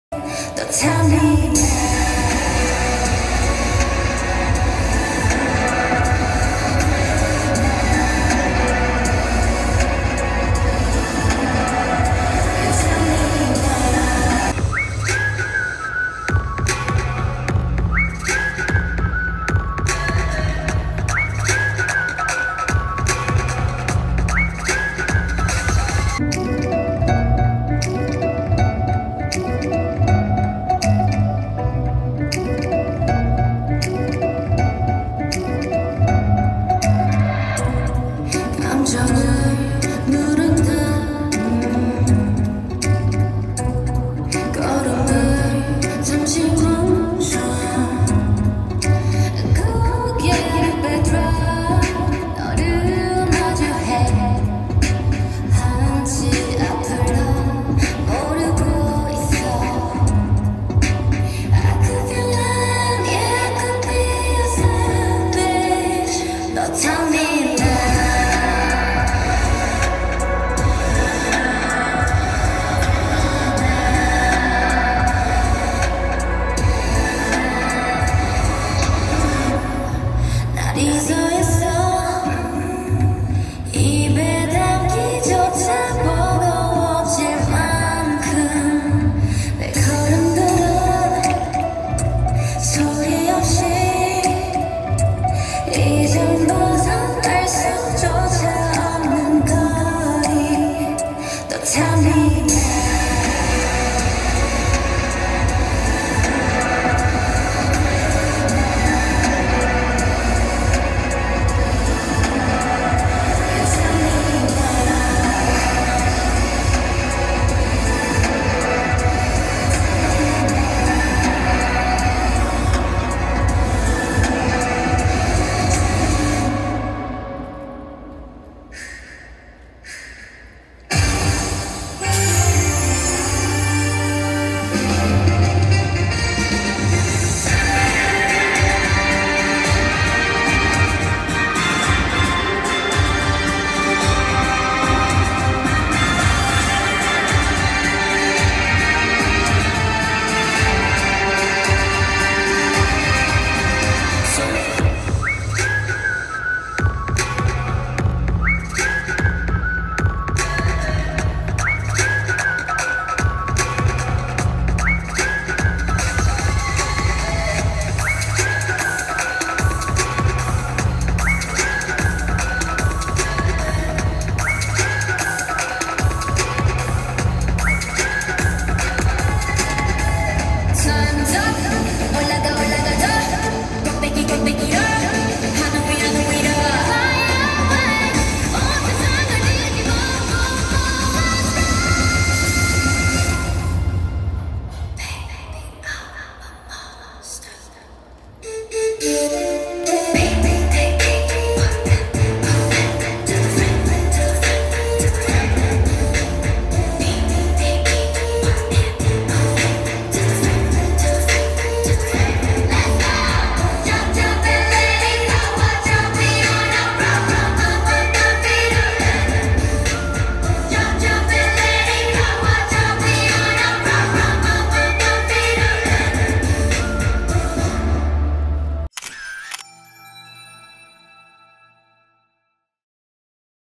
韓籍外援 安芝儇 性感辣跳「拐杖舞」超美舞蹈球迷全嗨翻！｜三立娛樂星聞
★訂閱【女孩要幹嘛】甜寵浪漫戲劇看到飽